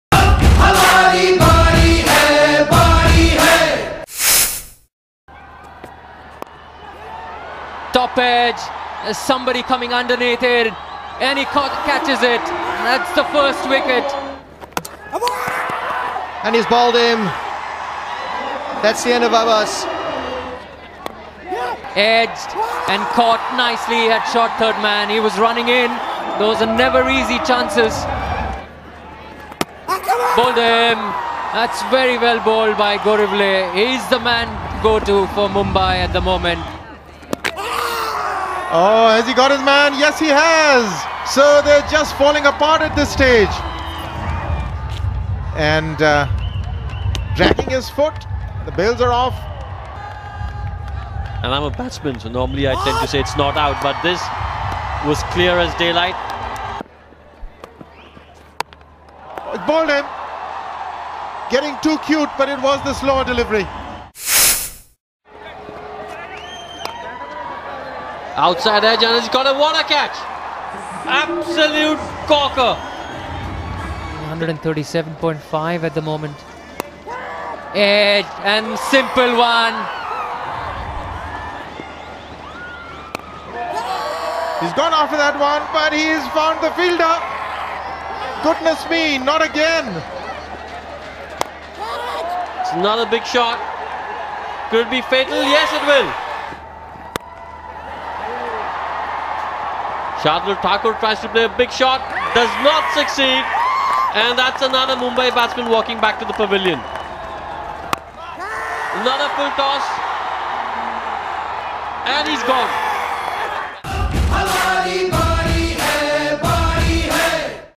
Mumbai University vs Jain University: Fall of Wickets
Mumbai spinners Pankaj Jaiswal and Sagar Gorivale had identical figures of 4 for 26 as Bengaluru finished at 145 for 6. But the star of the show was Bengaluru's left-arm spinner Suchith Jagadeesh who took 4 for 10 to ensure that Jain University won by six runs.